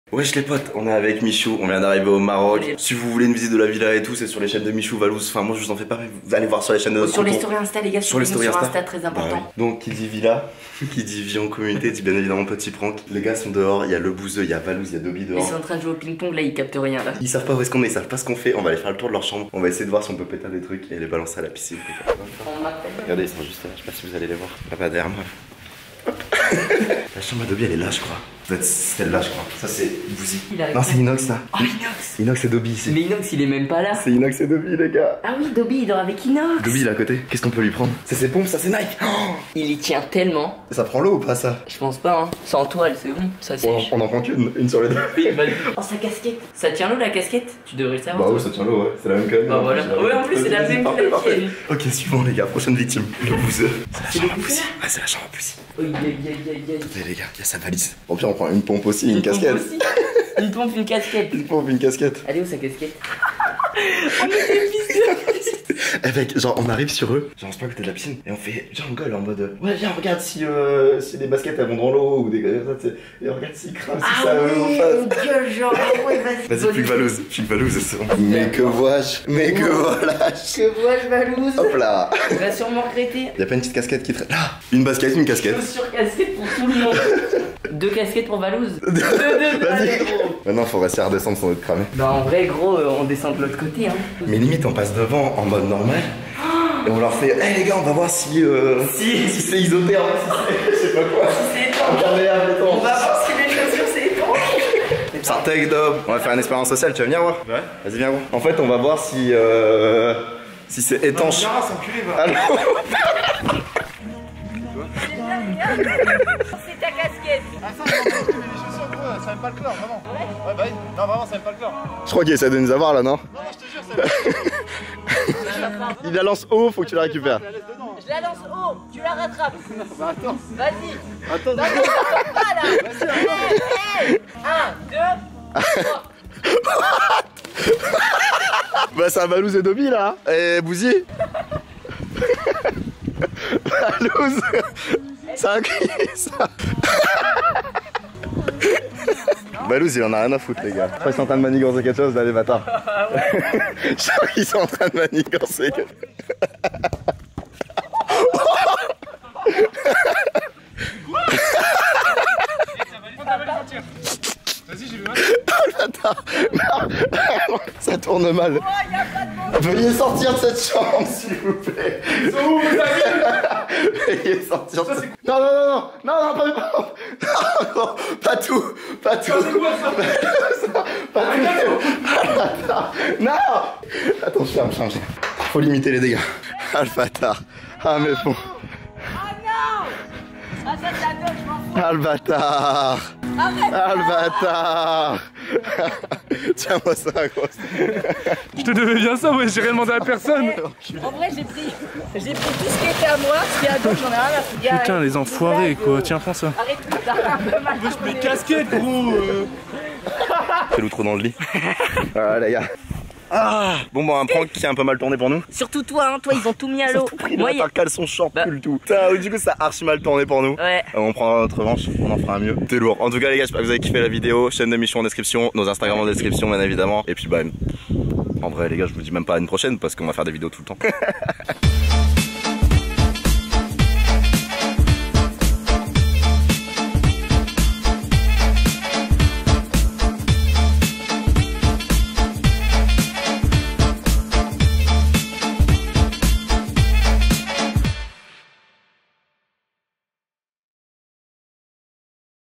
ON PRANK LA TEAM CROÛTON AVEC MICHOU POUR LE PREMIER JOUR A LA VILLA MAROCAINE
👉 Code créateur : Doc-Jazy

Wesh les potes, j'espère que vous allez bien. Comme la plupart savent déjà on est au Maroc avec la Team Croûton. Petite vidéo histoire de vous partager quelque moment que l'on passe avec la team! J'espère que vous kifferez cette vidéo !